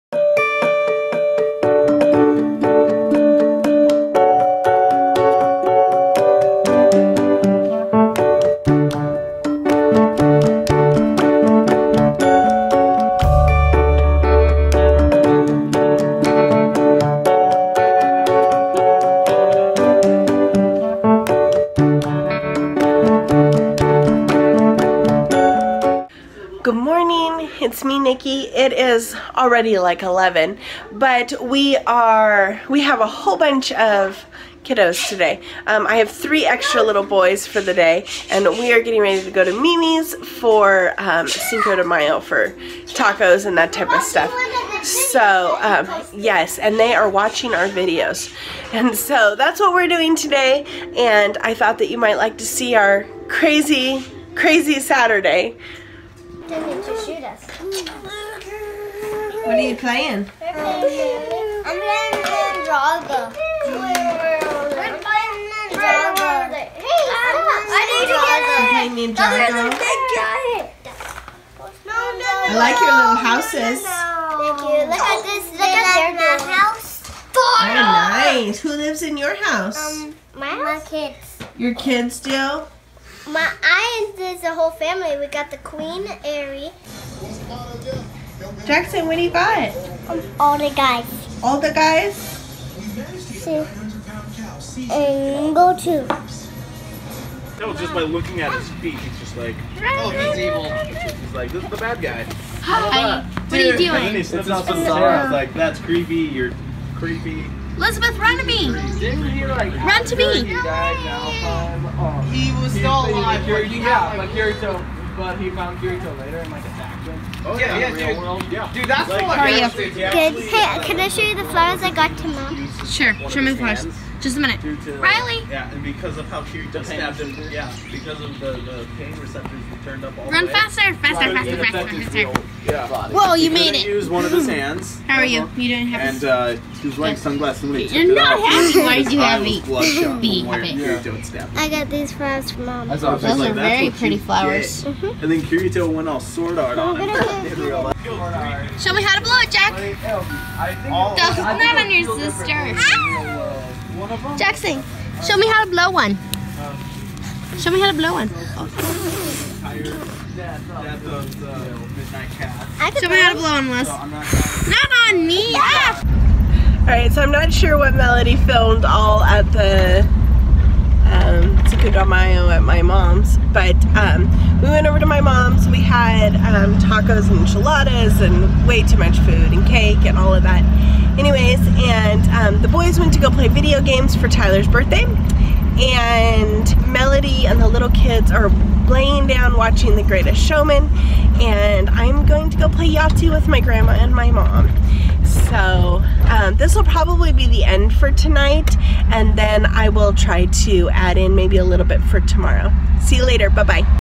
Cinco de Mayo
We got together at Mimi’s for a cinco de mayo lunch.